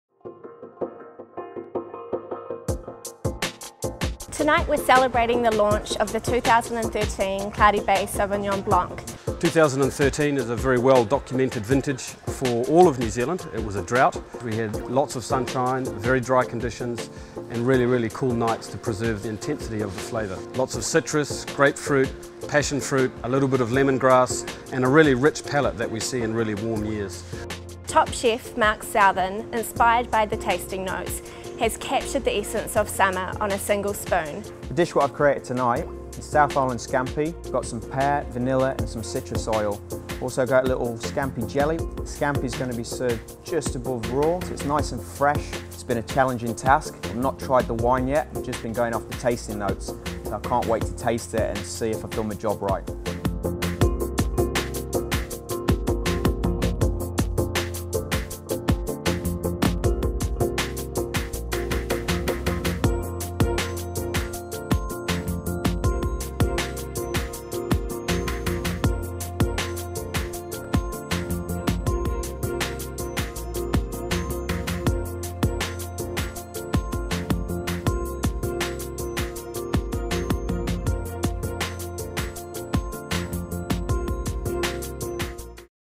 Cloudy Bay First of the Season 2013
The 2013 vintage of Cloudy Bay's international benchmark wine is now available to fine wine lovers worldwide.  An outstanding summer with excellent growing conditions has enabled Cloudy Bay winemakers to craft New Zealand's most precious summer flavours into their Sauvignon Blanc 2013 vintage.

Top chef Mark Southon was invited by Cloudy Bay to capture the essence of this First of the Season vintage in a dish created on a single spoon. The challenge for Mark Southon was to create a dish matched to the sauvignon blanc without having tasted it, basing his creation only on the tasting notes. The result was poached South Island scampi with pear, vanilla and citrus oil.